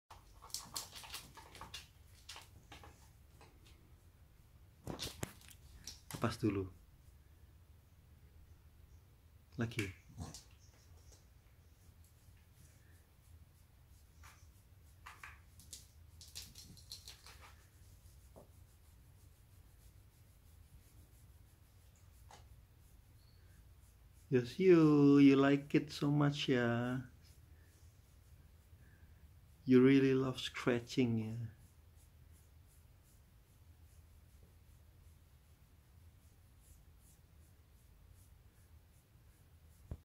Beagle loves a good back scratching
This is Yoshio, a beagle who really enjoy a good back scratching.

asking for more when we stop scratching him.

what a cute beagle...